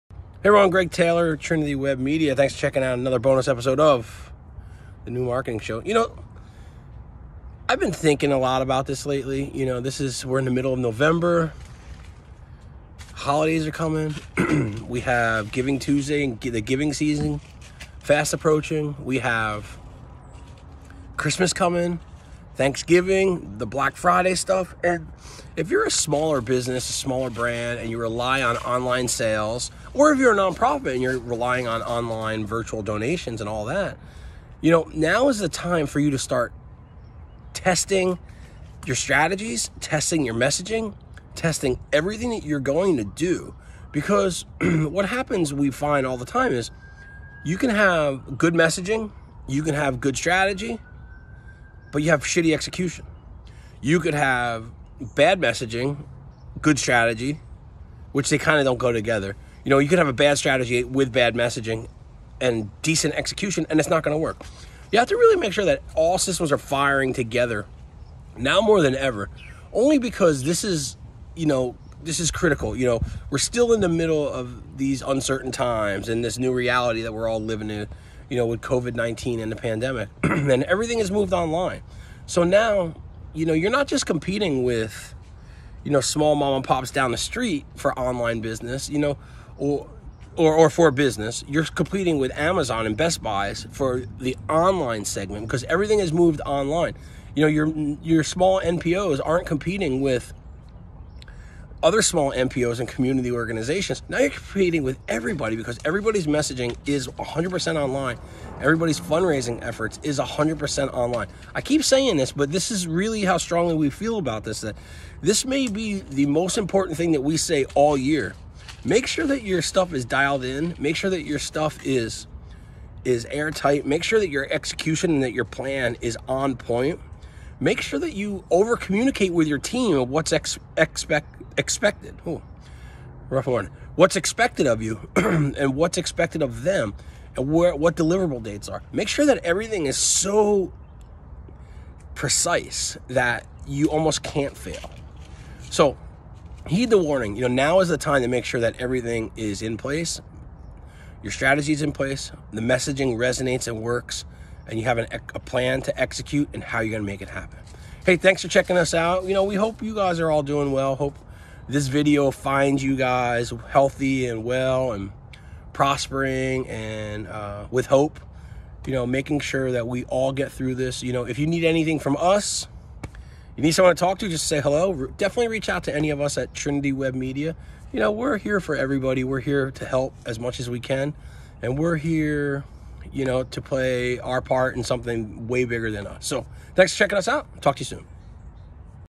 Small Business Advice for the Holiday Season
As we mentioned before, small businesses & NPOs will be most affected during this holiday season. With corporate entities getting involved in Giving Tuesday & other fundraising events, getting your strategy together has become more imperative than ever before. As a small business, what you must begin with is a strategy. Next on your list is finding ways to execute that strategy to its fullest potential. As we say, a plan without execution is nothing.

So, as we head into this chaotic season, you need to make sure all aspects of your online businesses are on point. With everyone moving online, you no longer compete with the mom and pop shop down the street. Your competition is now Amazon and Best Buy, making executing your strategy as crucial as ever.

Heed our warning. Now is the time to make sure all of your plans are in place and ready to take on the vast competition.